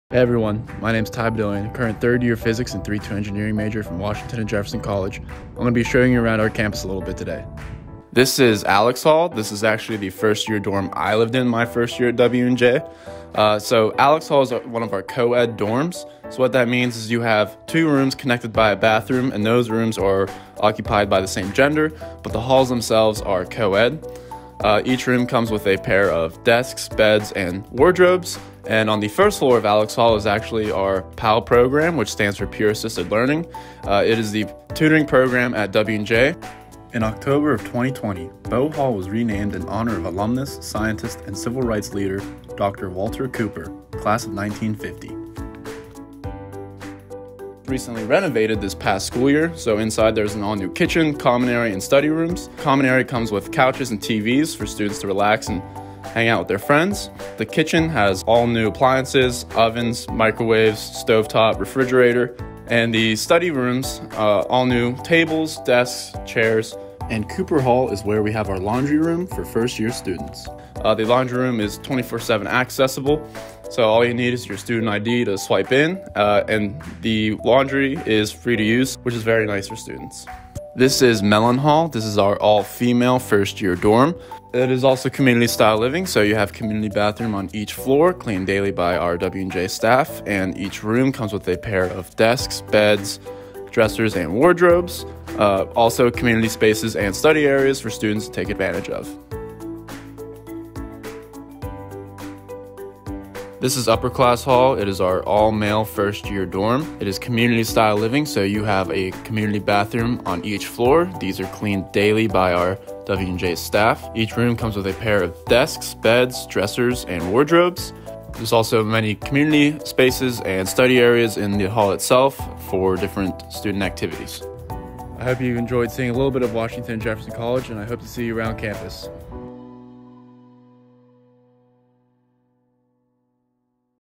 W&J College First Year Residence Halls Walking Tour with Ty Bedillion '21
Watch the video below to hear from Ty Bedillion, W&J
 Class of '21, as he takes you on a quick tour of the first-year
residence halls and introduces you to some of the first-year
programs that W&J's campus life has to offer you.

Ty Bedillion, Class of 2021

Connect with W&J!